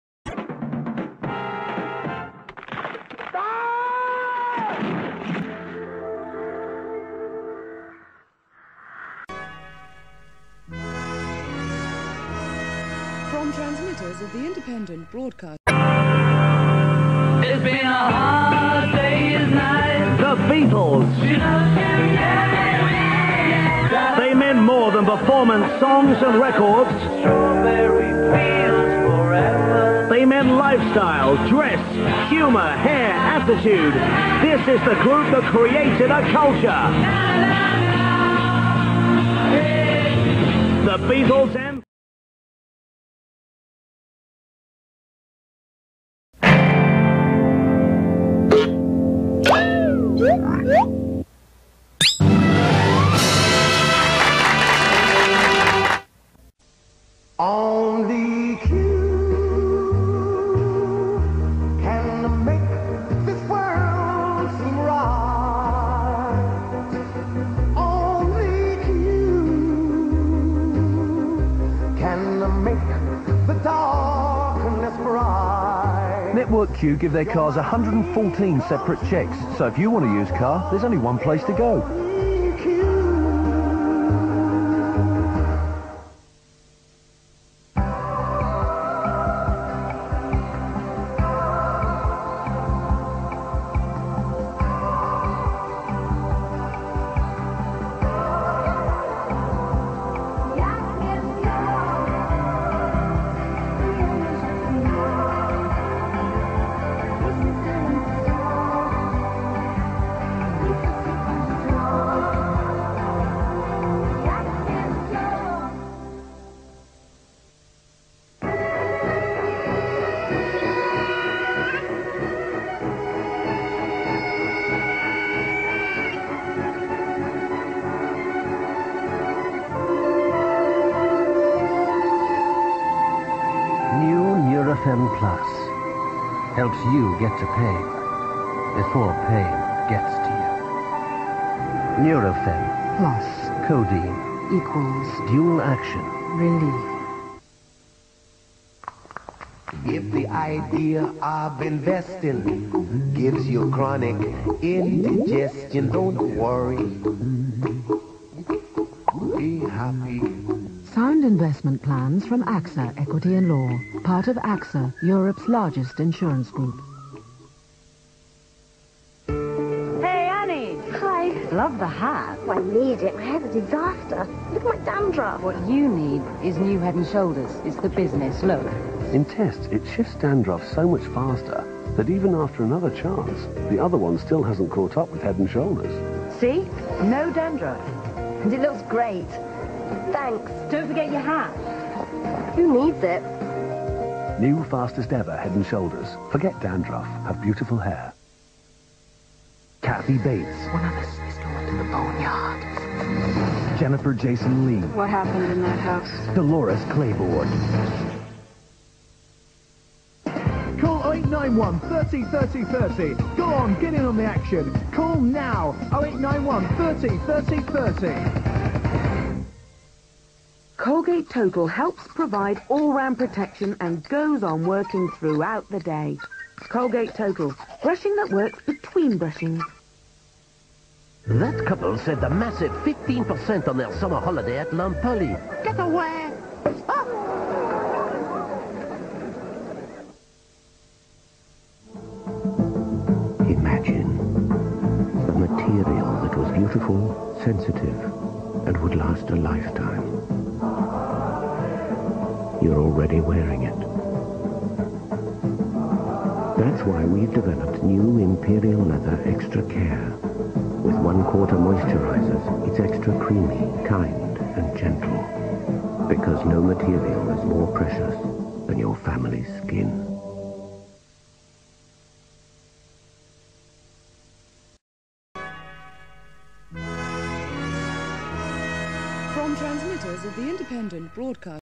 Nick at Nite UK Continuity 1995 - Part 3
Here's the latest batch of Nick at Nite UK Commercials. It's recorded from Torquay, England in October 1995.